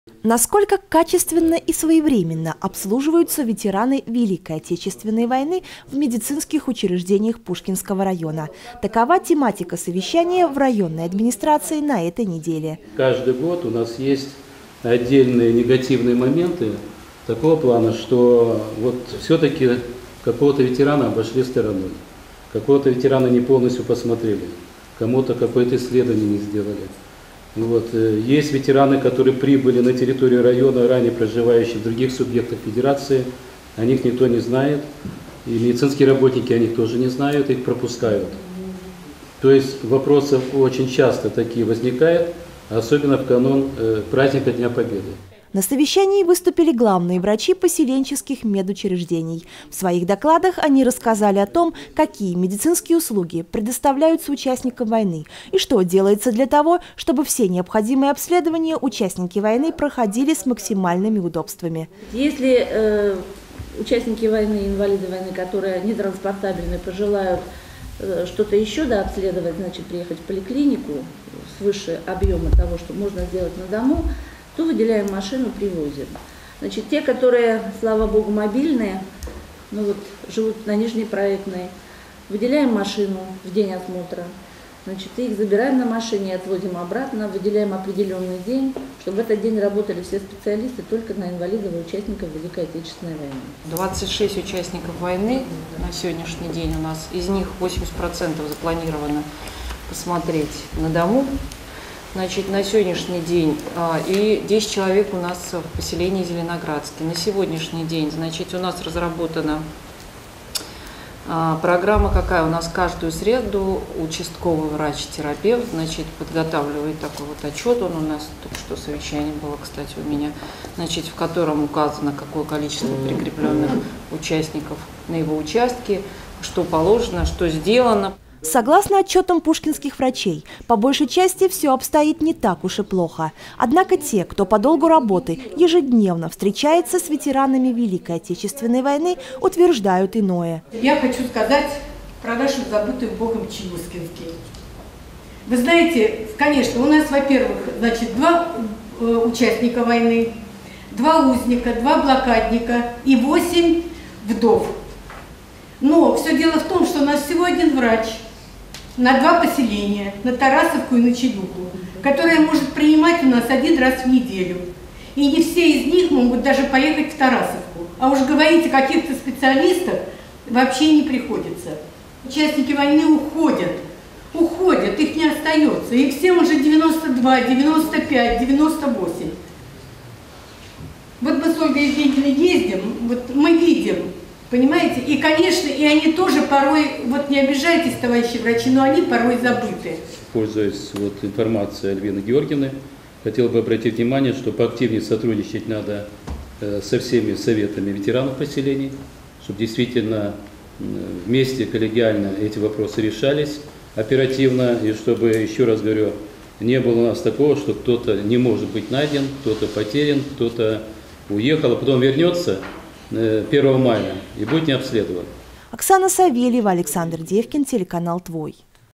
Медобслуживание ветеранов
О ветеранах Великой Отечественной войны и обо всех тех, кто так или иначе соприкоснулся с этой страшной войной — узниках фашистских концлагерей, жителях блокадного Ленинграда, тружениках тыла — мы вспоминаем, как правило, в преддверии празднования Дня Победы и других важных годовщин. А между тем, эти люди нуждаются в повседневном внимании и заботе. То, с каким медицинским обслуживанием и отношением медперсонала сталкиваются участники Великой Отечественной войны в Пушкинском районе, обсуждали на совещании в районной администрации на этой неделе.